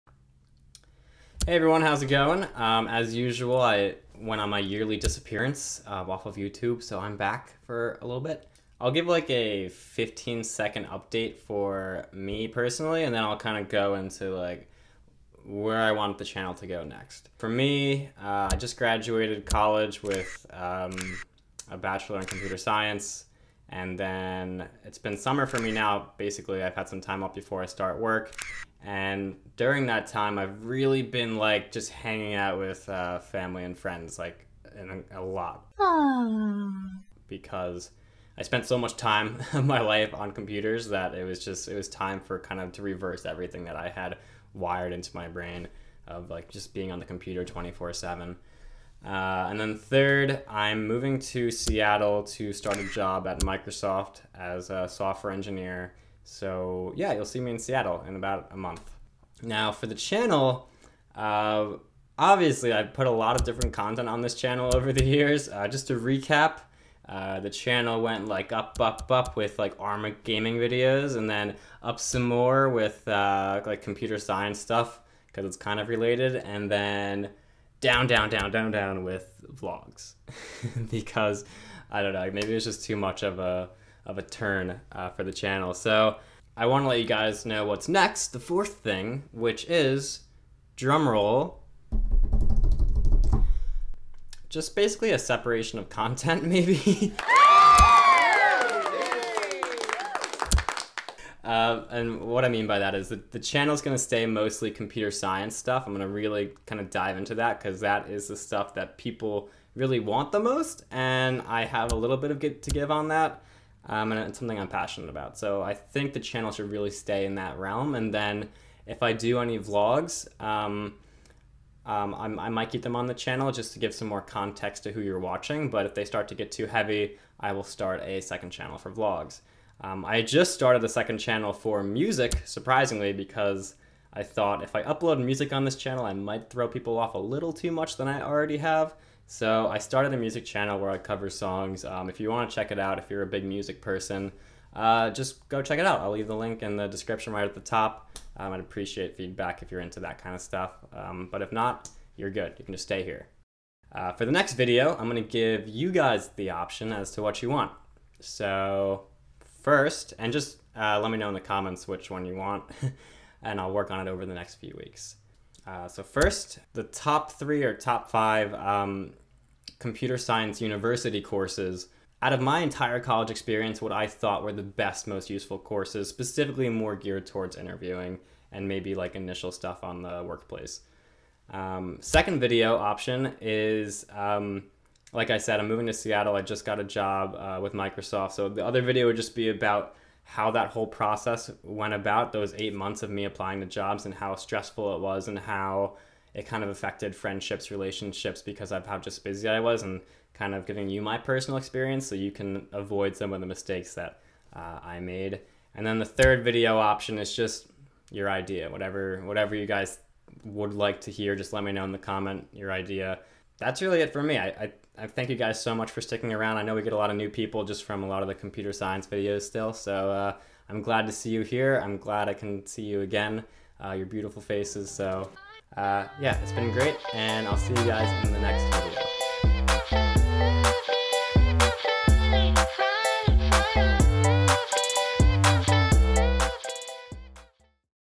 👋 2019! Personal & Channel Update. (CS Grad, Moving, New Channel)
In case anyone forgot... Bread == LCBradley3k

1. Best university courses to take that help with Computer Science job interviewing
2. Reflecting on 8 months of applying to jobs before joining Microsoft
3. Your idea! Let me know in comments.

Another update video straight from my bedroom after a long 9 month disappearance from YouTube. My Channel has gone from Arma 3 gaming videos, to Unturned gaming videos, to Computer Science videos, to vlogs. Lot of crazy stuff. I've given my audience a lot of turns, and I think it's time to settle down this channel into mainly Computer Science.

If I decide to try out other content types (like vlogs) and stick with them heavily, I will make a separate channel for it. For now the only separated channel I have is a music channel. I'd really appreciate feedback on the music covers if you would like to.

For now, I don't plan to be gaming that much, but I'd love to answer any questions you have regarding Computer Science at University/College, or Computer Science Careers.